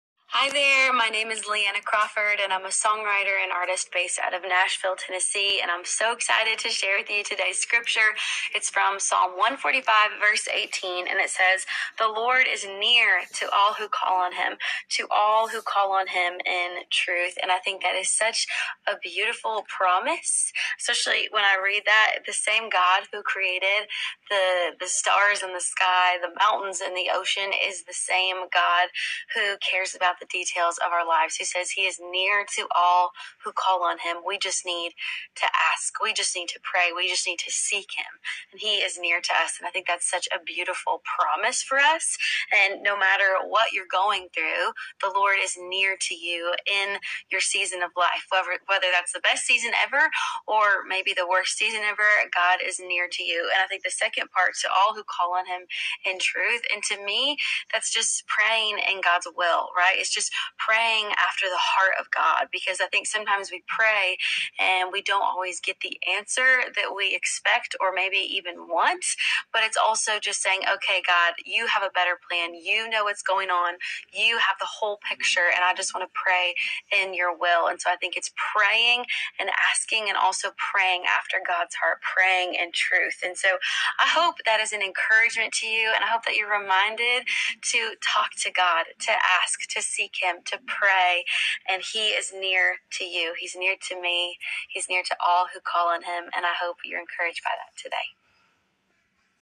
God is close to you _Leanna Crawford (YouVersion® Devotional, Psa. 145:18)
‭Psalms‬ ‭145:18‬ ‭NLT‬

The Lord is close to all who call on him, yes, to all who call on him in truth.

~ God is close to you, just talk to him.